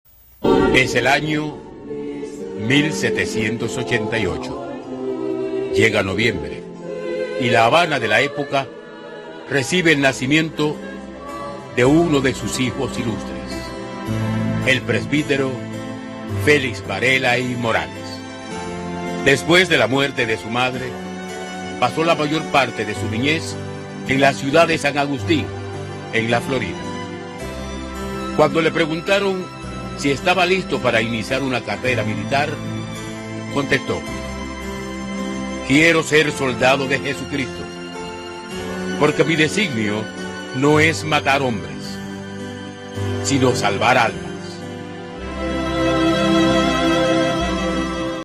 Del Padre Félix Varela, Cuba aprendió su pensamiento electivo, liberador y creador
FélixVarela fue el primer pensador cubano que planteó la necesaria separación de España, la lucha por la independencia y una radical modernización de la enseñanza en el país- El Padre Varela además fundó El Habanero, primer periódico independentista cubano. Fue un hombre que se definió a muy temprana edad por la claridad y madurez de su pensamiento.